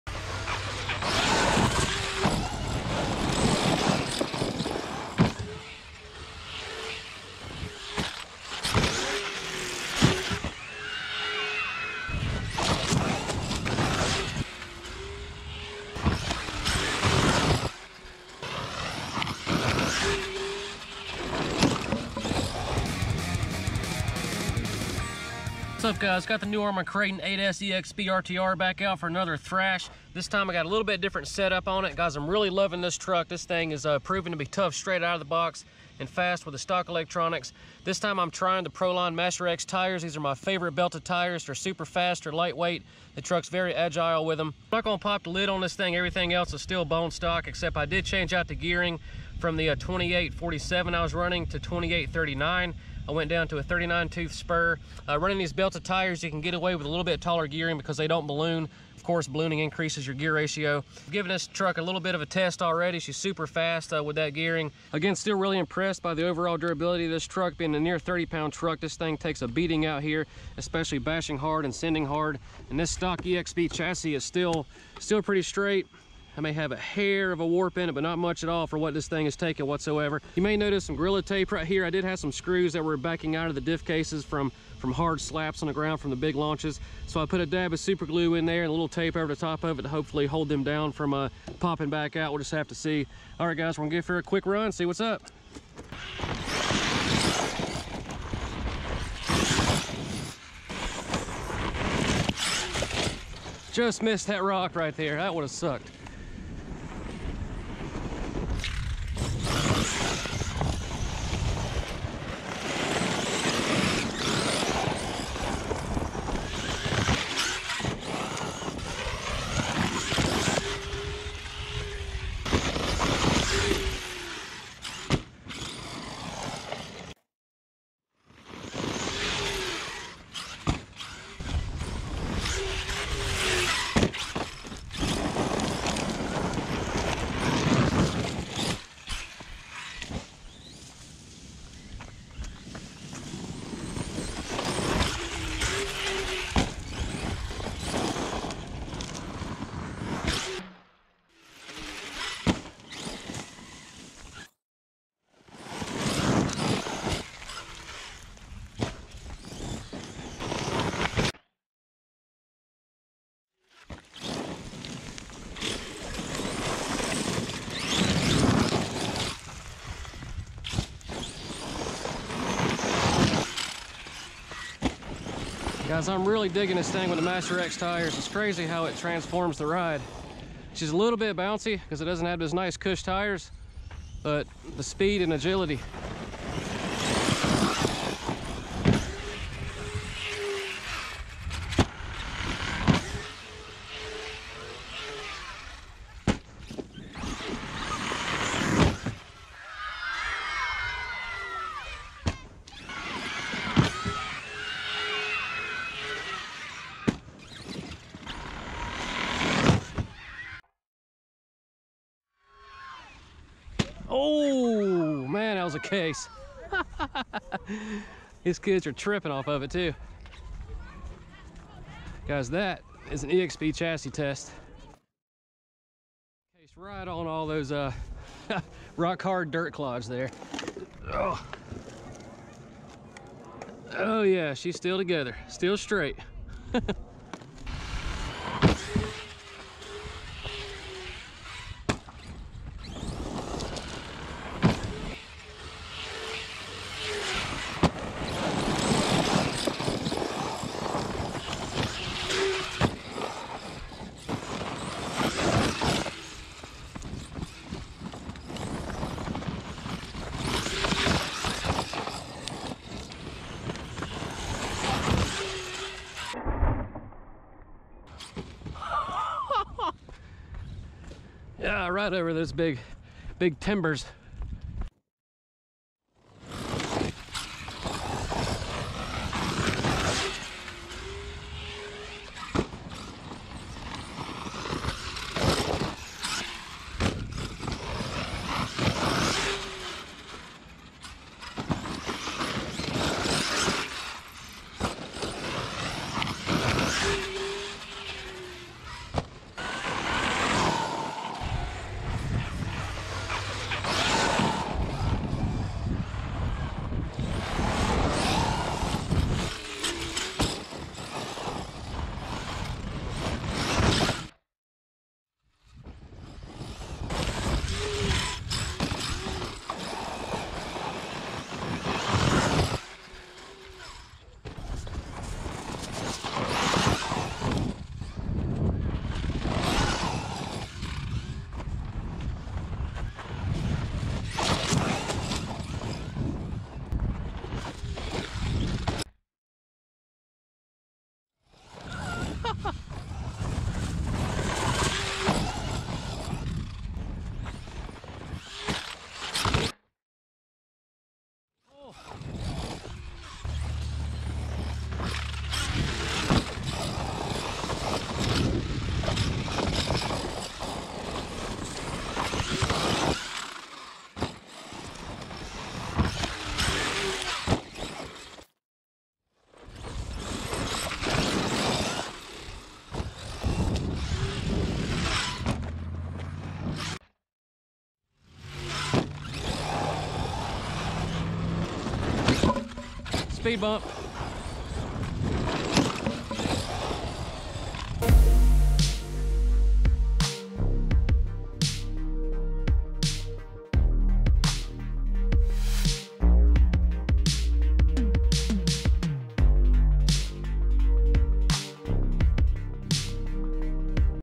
ARRMA Kraton 8s EXB with a NEW Brutally FAST Setup!! Extreme Beatdown!
Out with the New Arrma Kraton 8s EXB RTR with a new Brutally fast setup! Simply running stock electronics with Proline Masher X belted tires and 28/39 gearing! Really got her moving with decent temps! Had a total blast at both stops and only breaks were a wing mount and broken Motor lead in the ESC. I'll get her fixed back and ready to rip soon! HUGE thanks again to Arrma and Horizon Hobby! This is really a dream beast right out of the box!